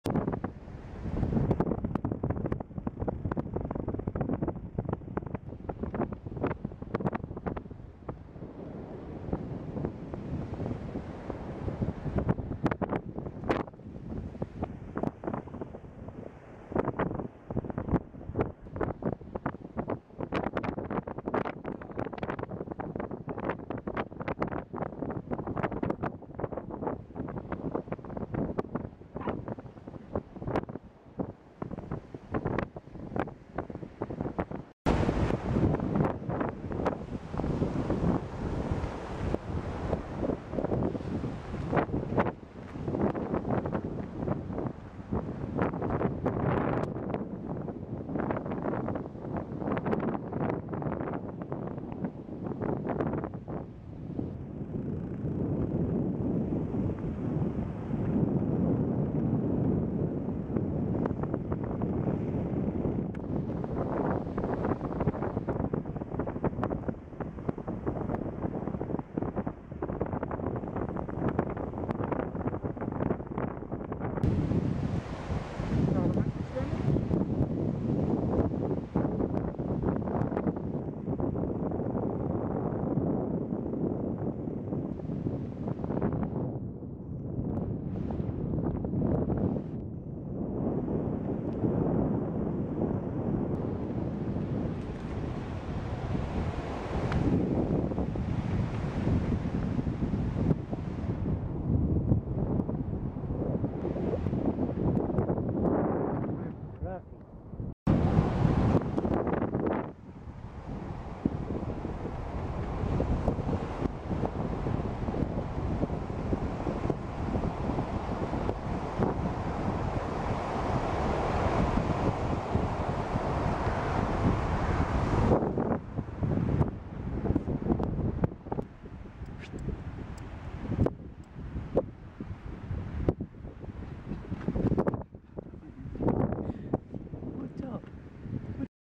Chester the Manchester Terrier having fun on the beach of Ameland
August 2013